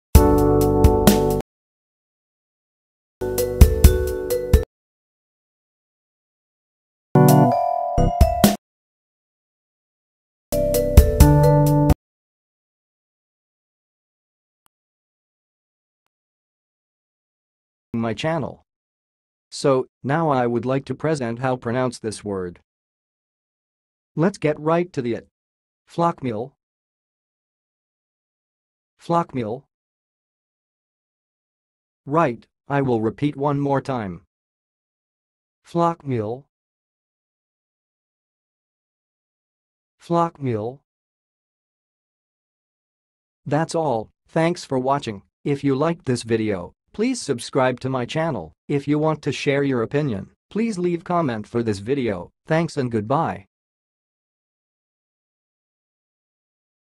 FLOCKMEAL - HOW TO PRONOUNCE FLOCKMEAL?
FLOCKMEAL - Flockmeal: What it is and how to pronounce it correctly in 2023

Pronunciation:
The pronunciation of 'flockmeal' can vary depending on personal preference since it is a made-up term.
However, a possible pronunciation is 'flock-meal', with the emphasis on the first syllable in 'flock'.
Alternatively, it could be pronounced as 'flock-mill', with the emphasis on the second syllable in 'flock'.
It's important to note that since this term is not commonly used, different people may have different interpretations of its pronunciation.

Test yourself by speaking the following examples:
1. The annual flockmeal celebration brings bird enthusiasts from all over the world
2. We had a delightful flockmeal by the lake, enjoying the company of various waterfowl
3. I have never heard of flockmeal before, is it some sort of bird-themed feast?